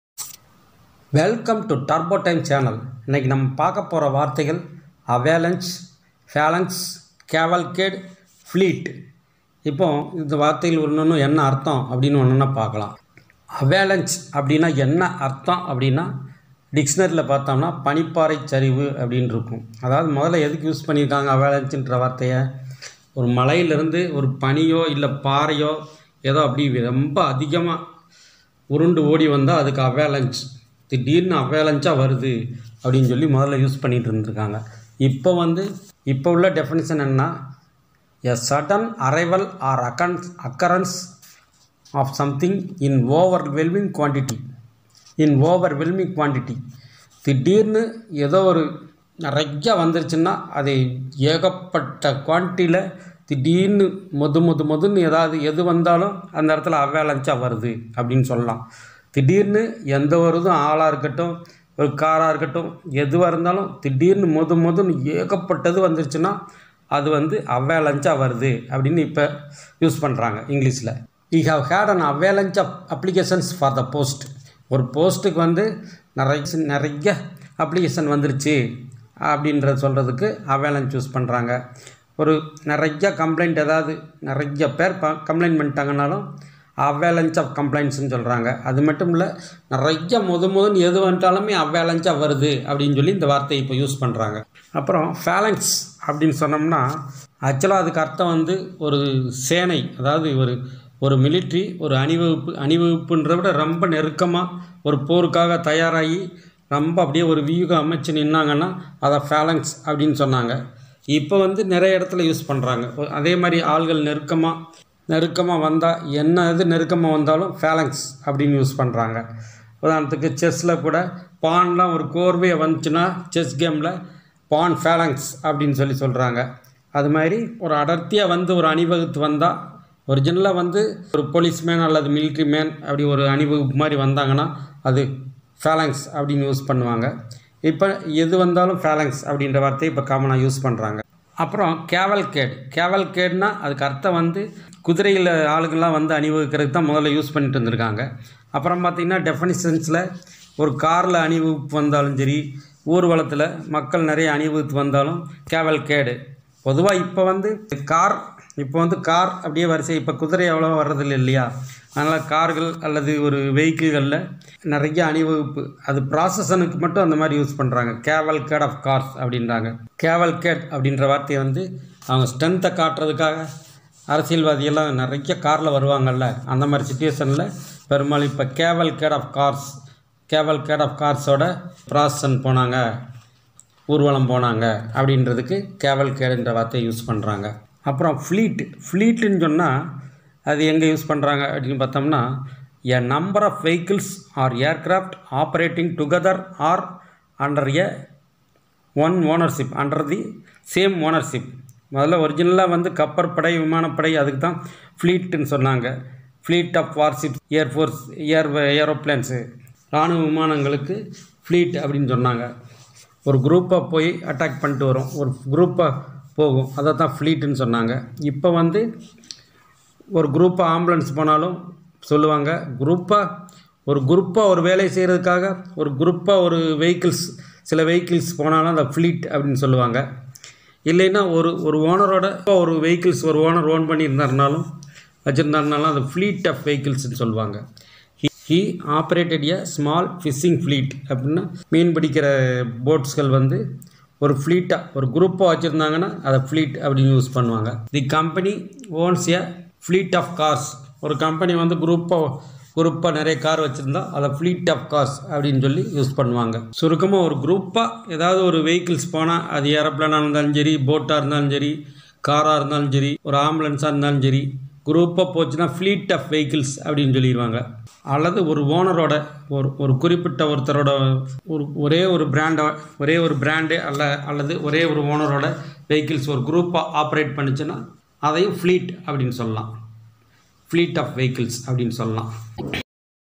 Avalanche, phalanx,cavalcade and fleet explanations in Tamil,improve your vocabulary
Avalanche, phalanx, cavalcade and fleet explanation in Tamil,
எளிமையான முறையில் தமிழ் வழி இங்கிலிஷ் கற்கலாம்,
Improve your English language grammar and vocabulary through easy Tamil